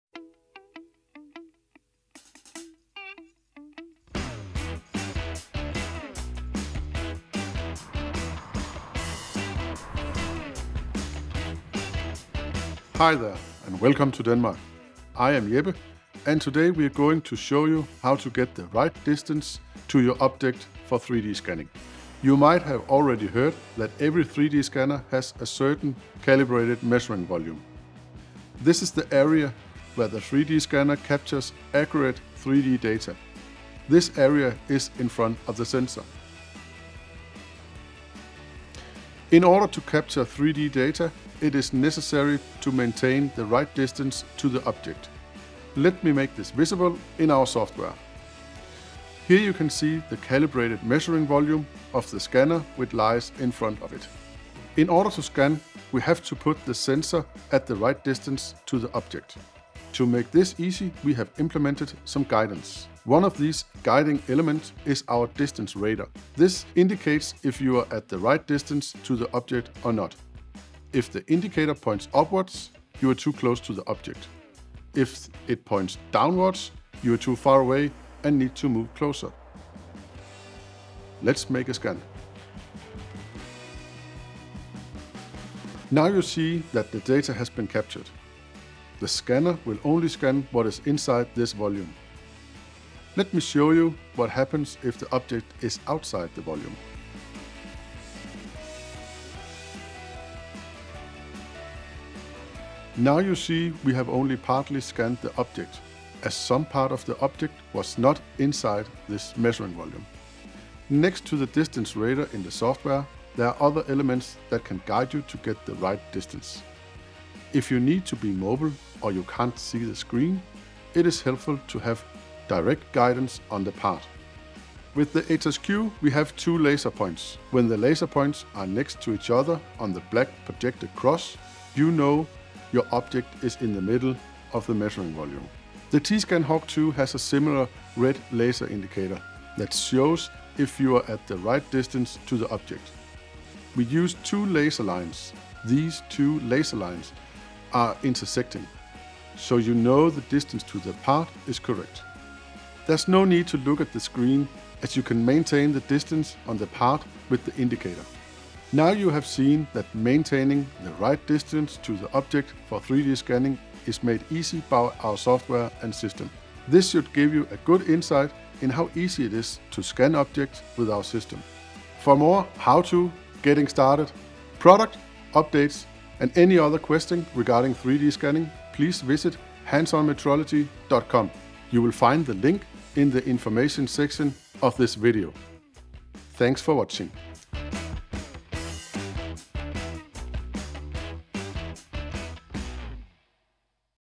How to 3D scan with the right distance to object
For 3D scanning the optimal distance to the objects is important for highly precise data and an easy 3D scanning procedure. Let Jeppe show you how to detect the right distance with the optical 3D scanner.